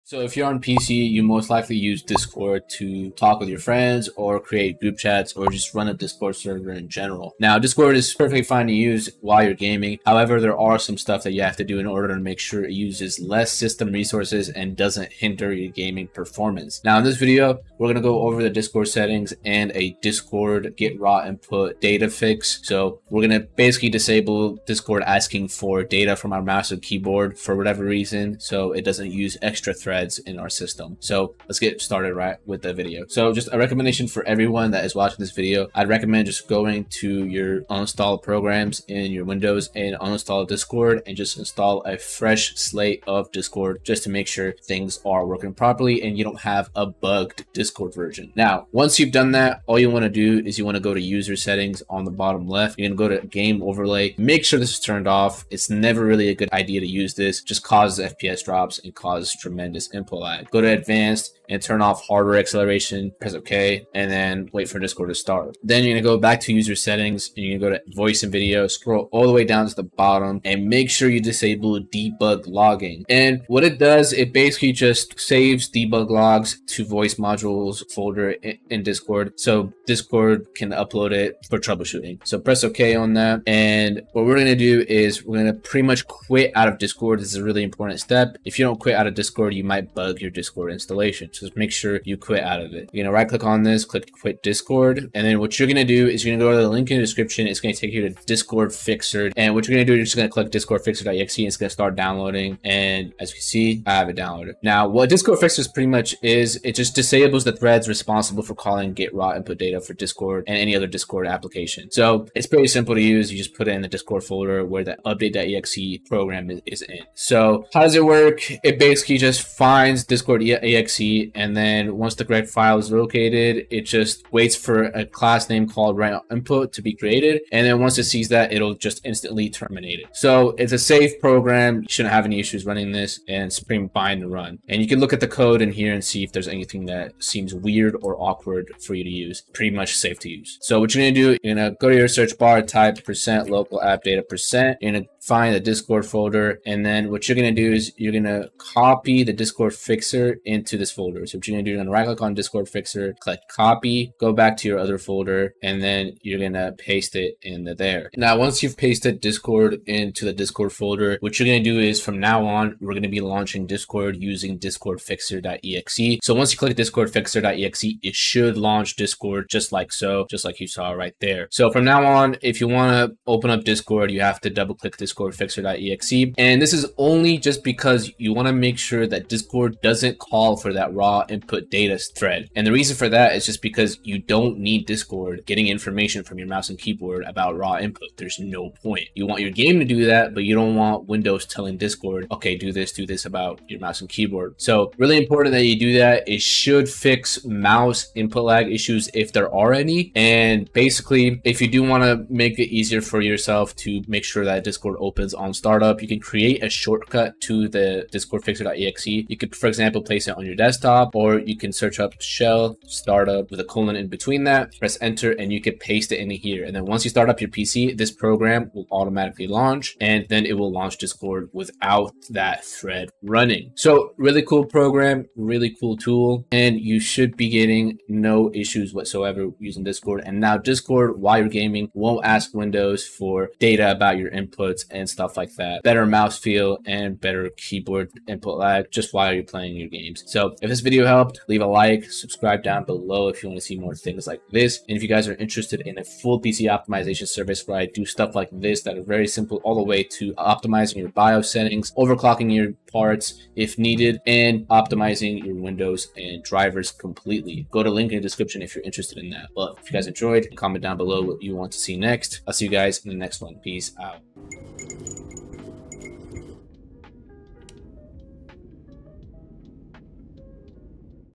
Fix Discord causing BAD GAME FEEL and LOWER FPS
In this video I'll be covering how to turn off useless settings in Discord, prevent Discord from creating a thread in your Windows that constantly asks for your mouse and keyboard raw input data by using Discord Fixer below.